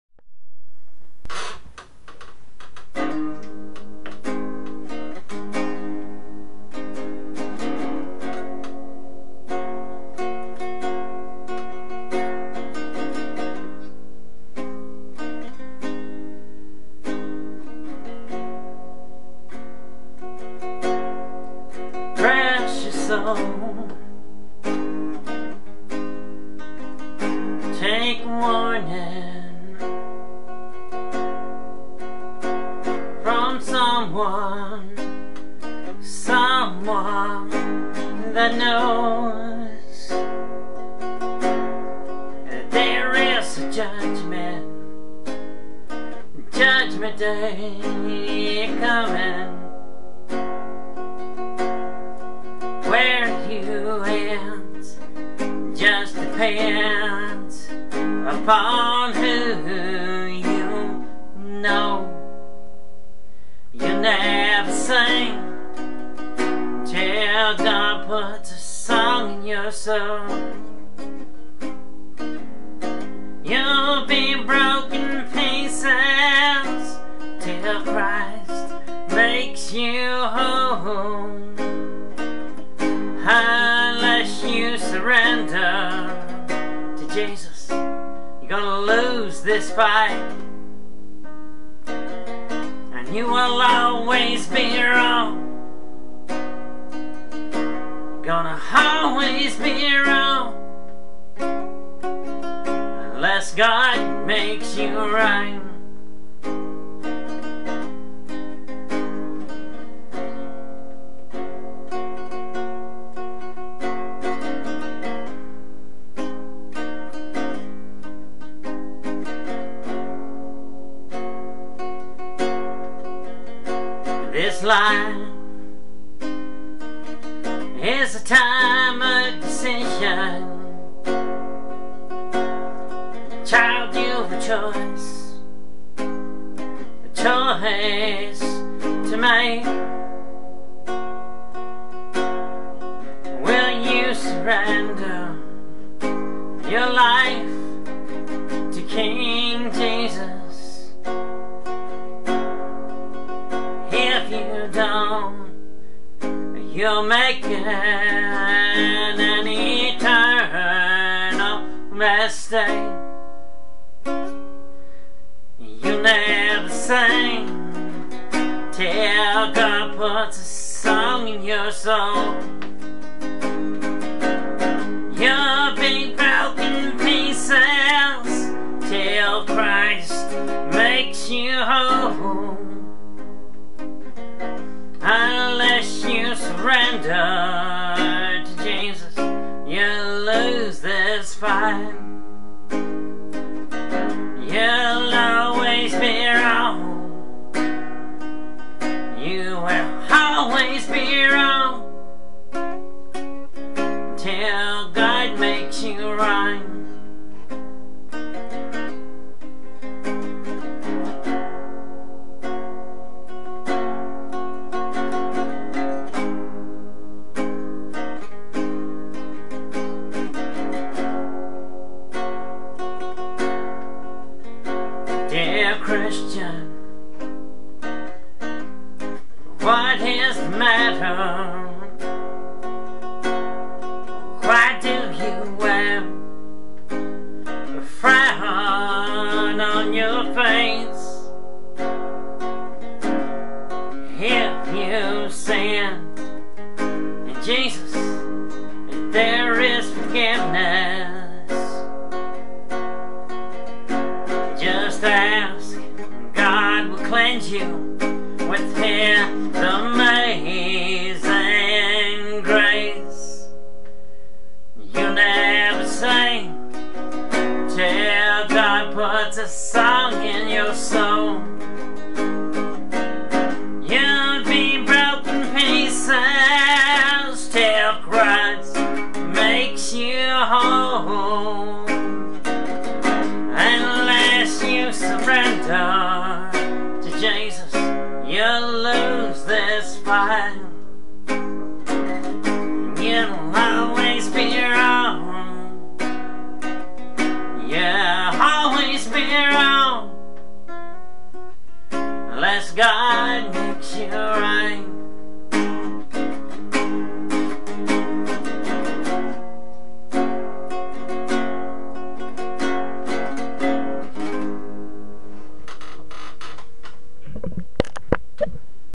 God Makes you Right
Though we can sing notes, they do not make music unless they praise our Savior, and we are broken records until His love makes us new.